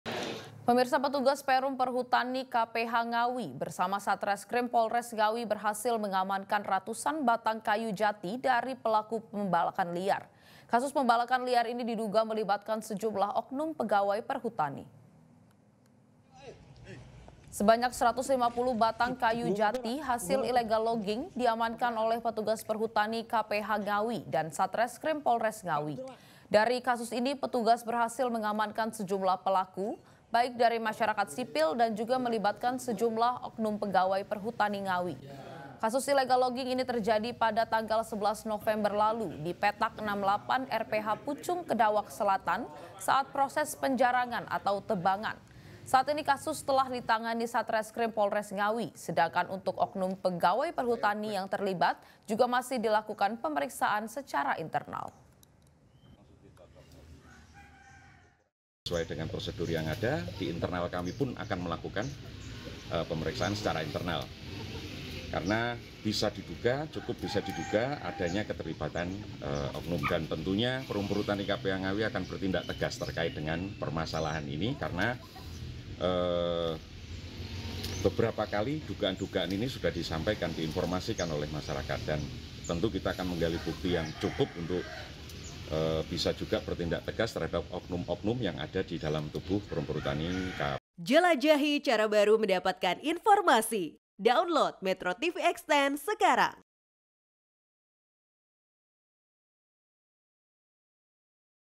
Pembalakan Liar, Petugas Amankan Ratusan Batang Kayu Jati
METRO TV JATIM, Petugas Perum Perhutani K-P-H Ngawi Bersama Satreskrim Polres Ngawi Berhasil Mengamankan Ratusan Batang Kayu Jati dari Pelaku Pembalakan Liar. Kasus pembalakan liar ini diduga melibatkan sejumlah oknum pegawai Perhutani.